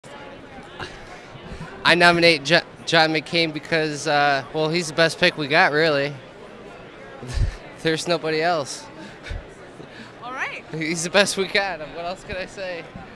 Robert Fisher: "I nominate John McCain because..."
An RNC attendee tells why they are nominating John McCain